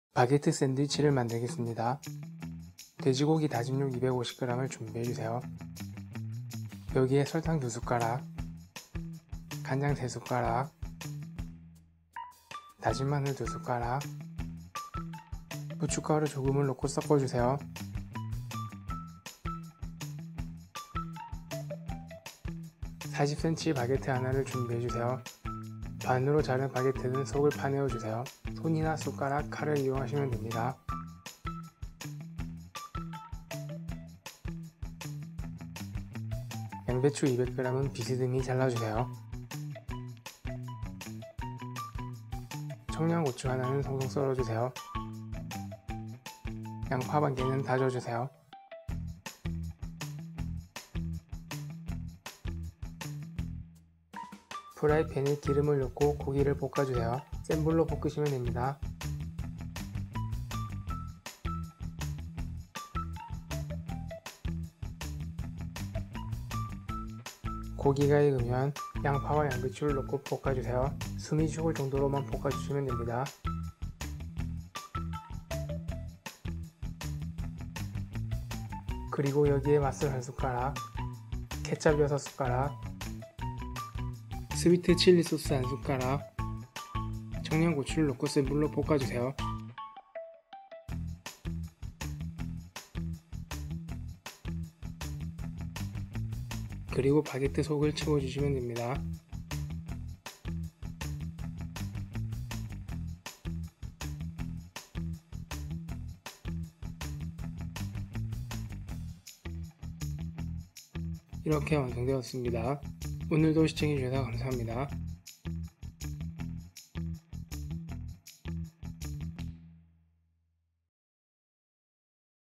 바게트는 매콤한 샌드위치로 드세요.
매콤함이 매력적인 바게트 샌드위치입니다.

* 재료 / ingredient *

돼지고기 다짐육 250g, 설탕 2숟가락, 간장 3숟가락, 다진 마늘 2숟가락, 후춧가루 조금, 바게트 1개 (40cm), 양배추 200g, 청양고추 1개, 양파 1/2개, 맛술 1숟가락, 케찹 6숟가락, 스위트칠리소스 1숟가락

*만드는 법*
1. 돼지고기 다짐육을 준비한다.
2. 다짐육에 설탕, 간장, 다진 마늘, 후춧가루를 넣고 섞는다.
3. 반으로 자른 바게트의 속을 파내어 준다.
4. 양배추는 비슷듬히 잘게 자른다.
5. 청양고추는 송송 썬다.
6. 양파는 다져 준다.
7. 팬에 식용유, 돼지고기 다짐육을 넣고 센 불로 볶는다.
8. 고기가 익으면 양파, 양배추를 넣고 숨이 죽을 정도로만 볶는다.
9. 야채의 숨이 죽으면 맛술, 케찹, 스위트칠리소스, 청양고추를 넣고 볶는다.
10. 볶아 두었던 재료를 이용하여 바게트 속을 채운다.

* 2023 by 요알남 MingStar. All rights are reserved.*